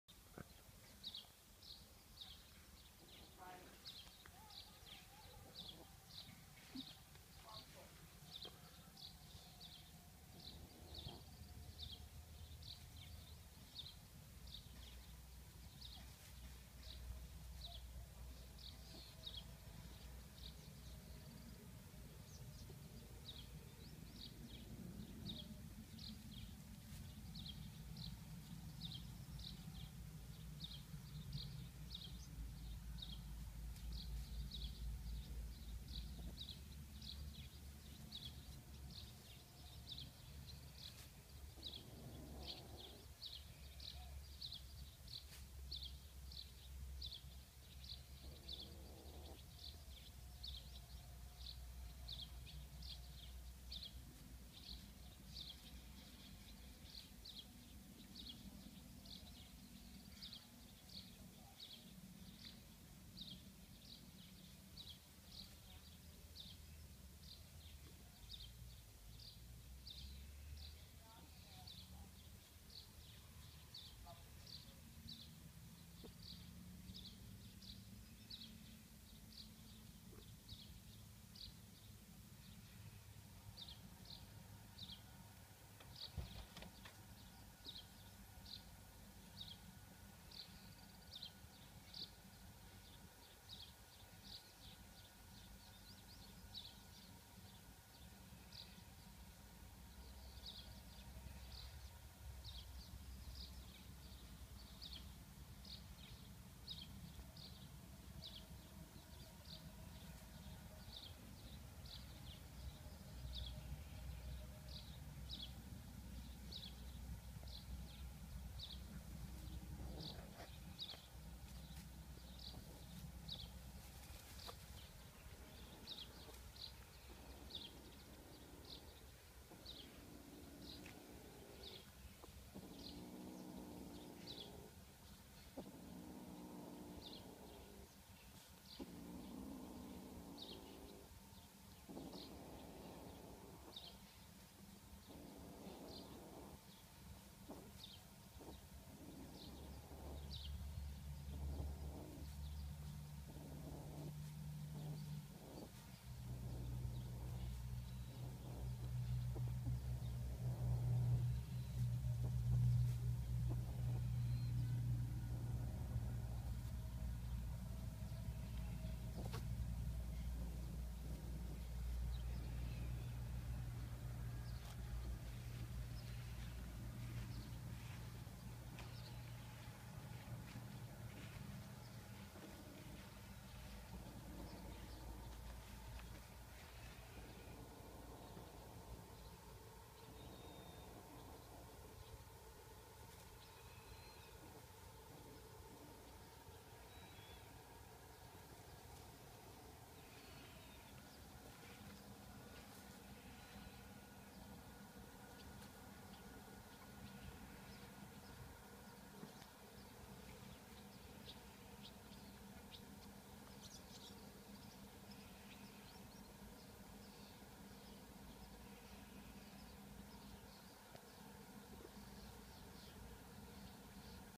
Cat and mouse play
Little cat play with mouse untill mouse get away.